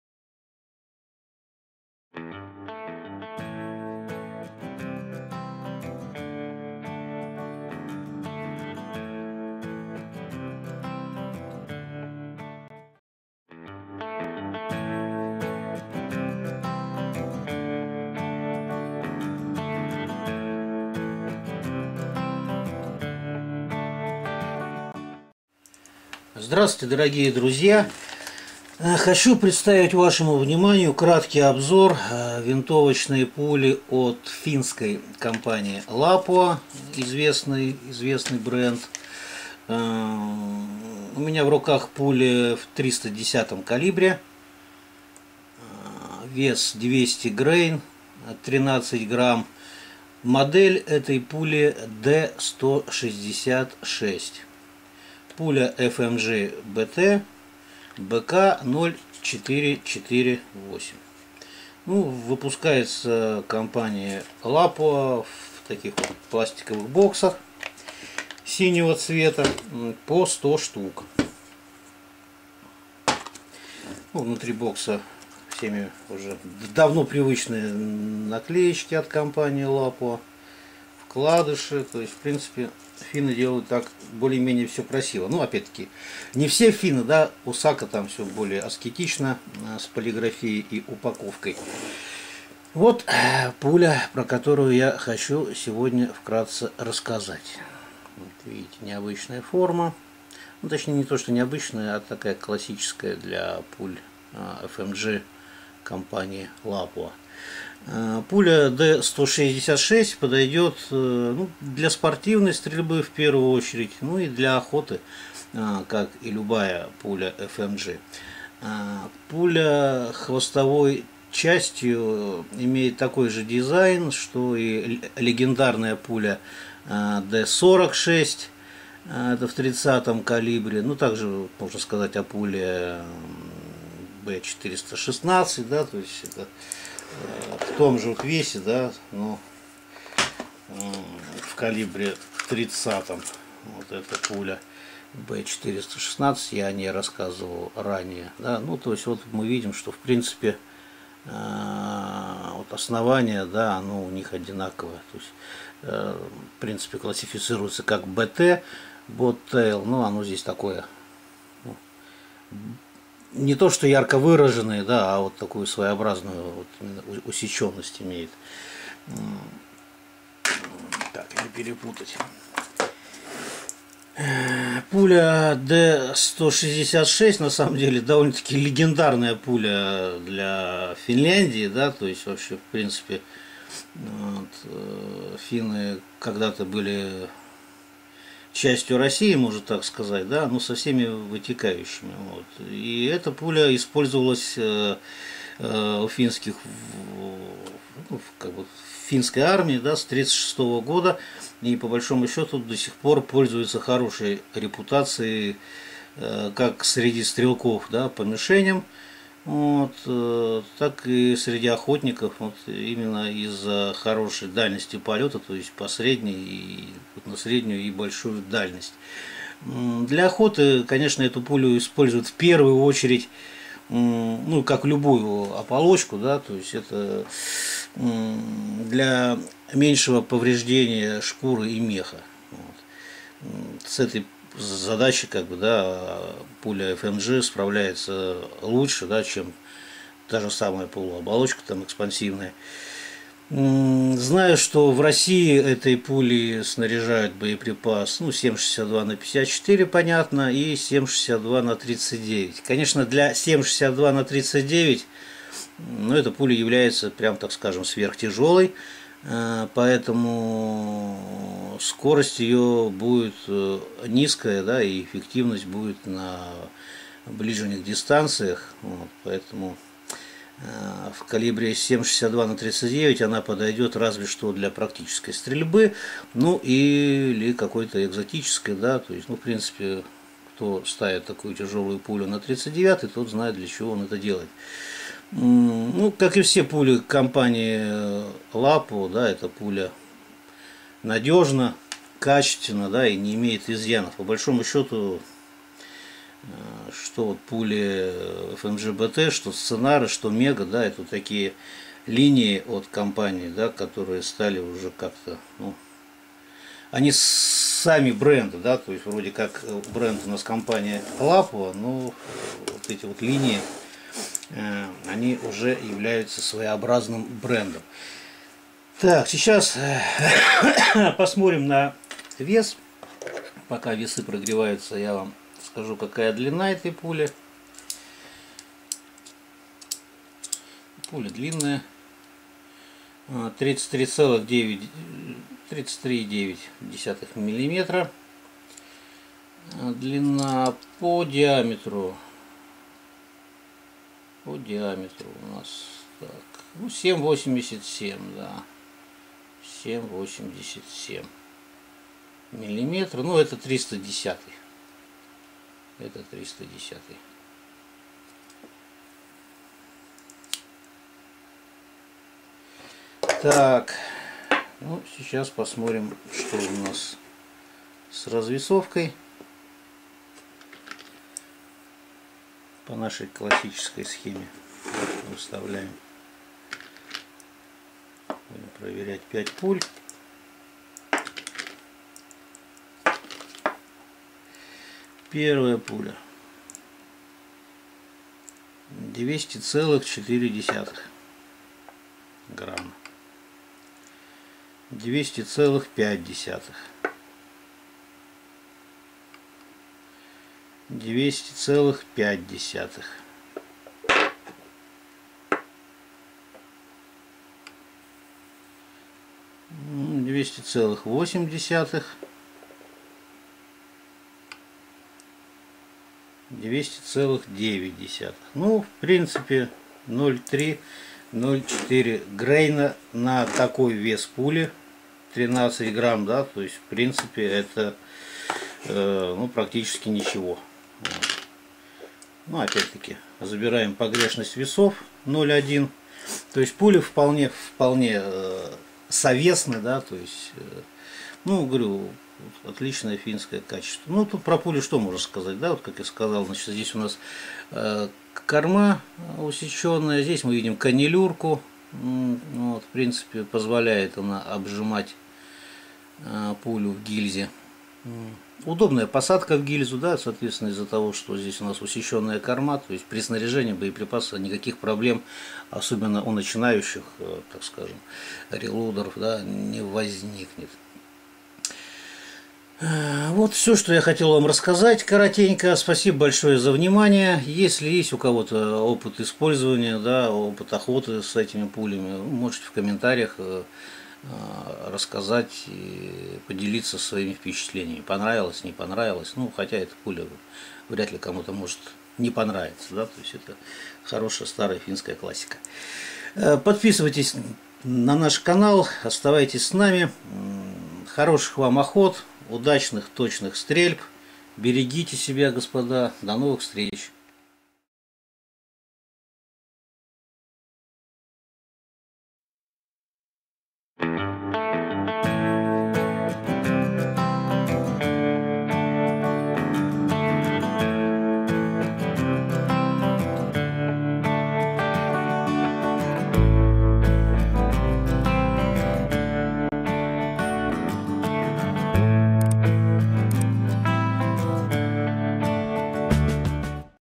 Lapua D166 .310 200gr/13,0 грамм, , FMJBT, ВС - 0,448
Обзор пули Lapua .310 200gr/13,0 грамм, D166, FMJBT, ВС - 0,448
Описание, внешний вид, характеристики.
ТОВАРЫ С ALIEXPRESS, КОТОРЫЕ МОГУТ ПРИГОДИТСЯ. ТОВАР И ПРОДАВЕЦ ПРОВЕРЕНЫ ЛИЧНО.